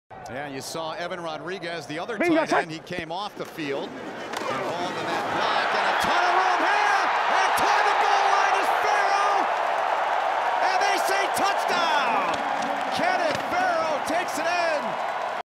Farrow Leaps Into End Zone for San Antonio Commanders
Check out highlights from The Alliance of American Football's Week 2 action, where the Orlando Apollos took on the San Antonio Commanders.